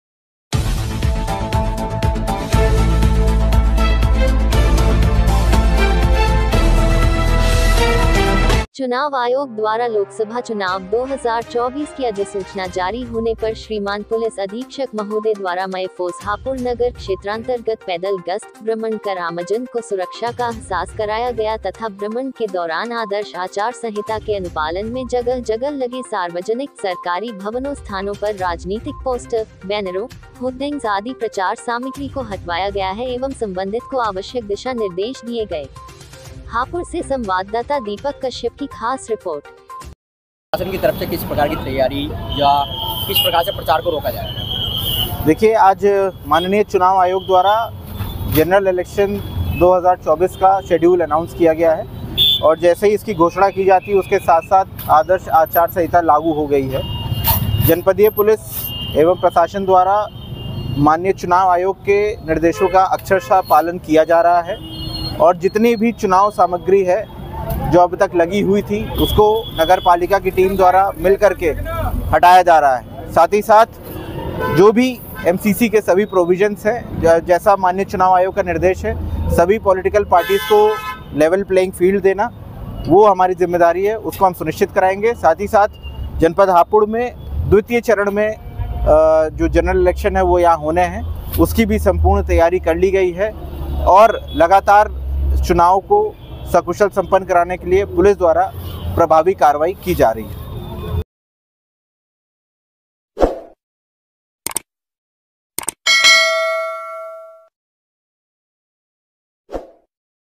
पुलिस अधीक्षक द्वारा मय फोर्स हापुड़ नगर क्षेत्रान्तर्गत पैदल गस्त की गई
hapur || पुलिस अधीक्षक द्वारा मय फोर्स हापुड़ नगर क्षेत्रान्तर्गत पैदल गस्त की गई